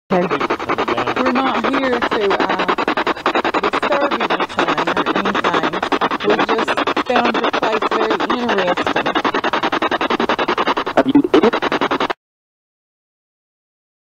Investigator gives a very nice compliment...Listen to the response.
Investigator makes the comment were not here to disturb anything we just found your place very interesting. A disembodied male voice is captured saying..."Oh you did"?